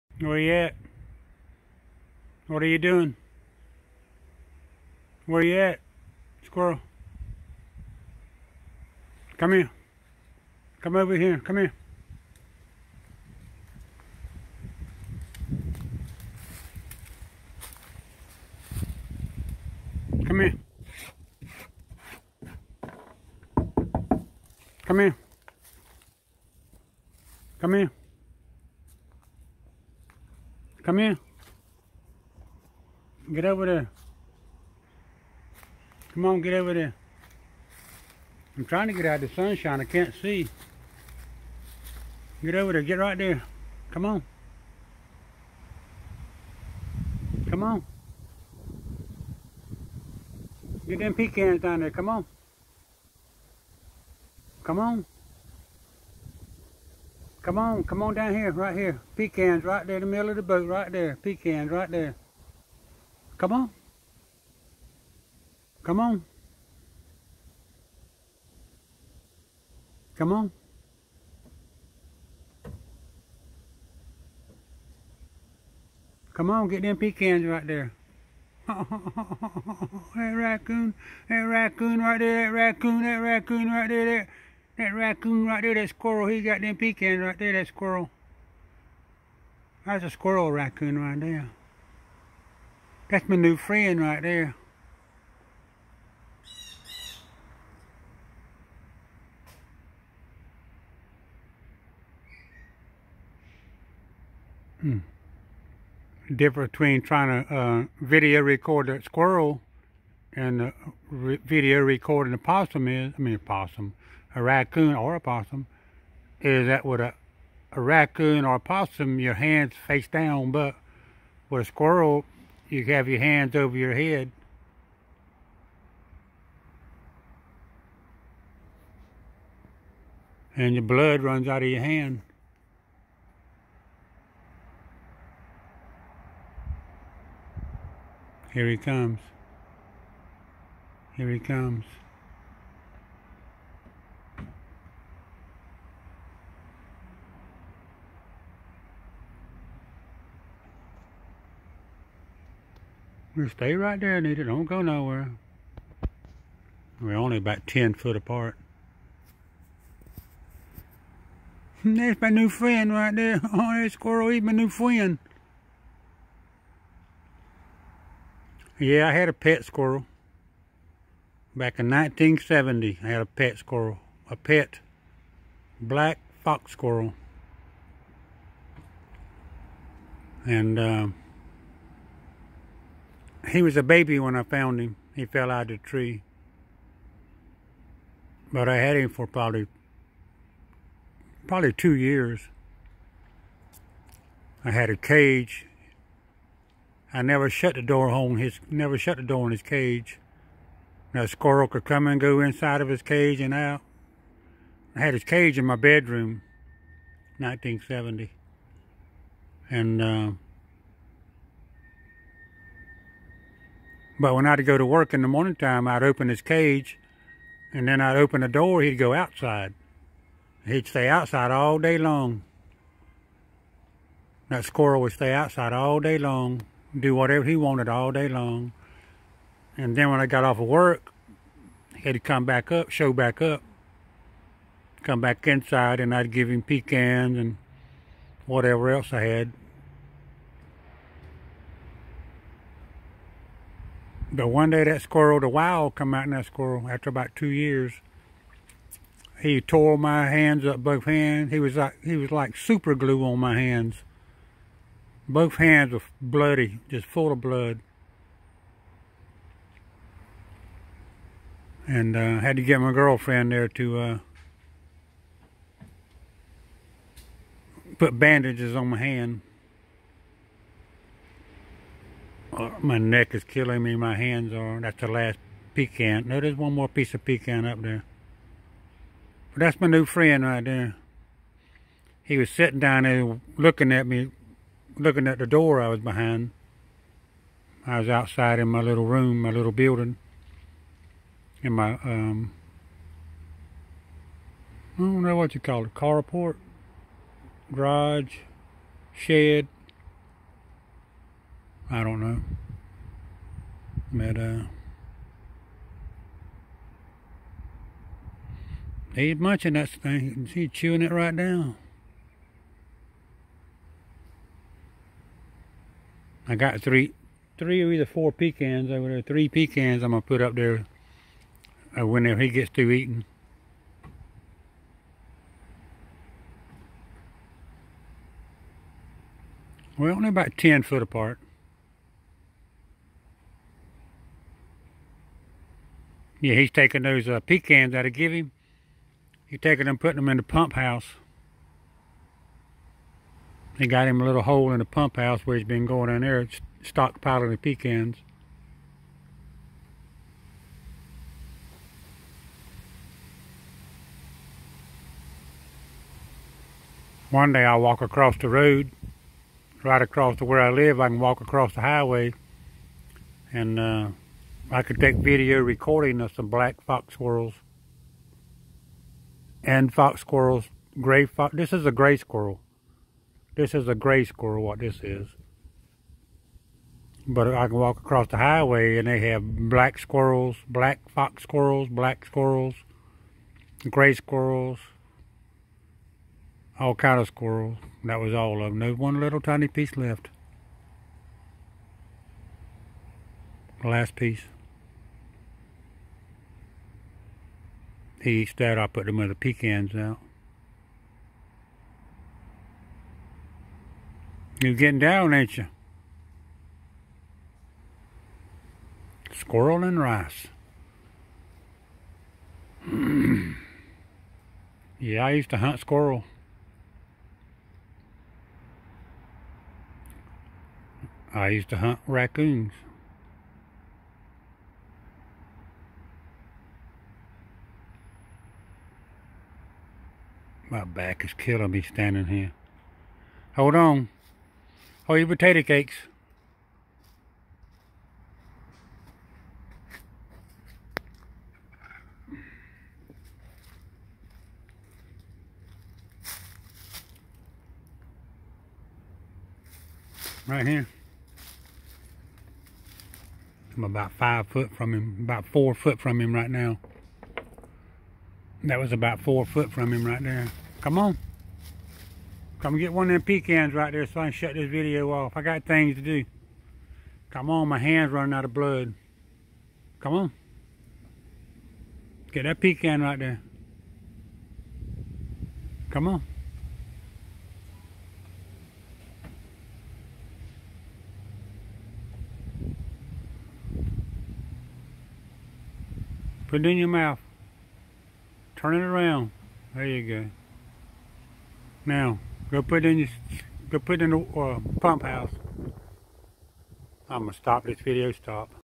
Squirrel Waiting on Me
This squirrel was sitting about 5 feet from my door just looking at it.. So I come outside and the said to the squirrel to come here & he listens to me.. he comes here.. a little nervous but not scared..